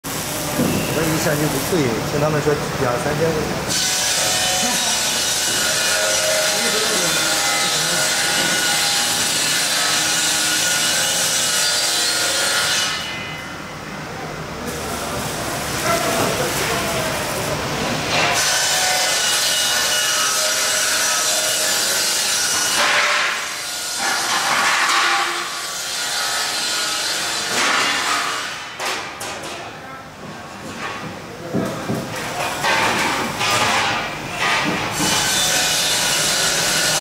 Powder spray booth filter recovery system
This cartridge filter recovery system is specially for our Canada customer. COLO design and manufacture various powder recovery system for spray booth. Details

We supply powder collection module for manual or automatic powder coating booth, filter systems and cyclone unit all can be designed.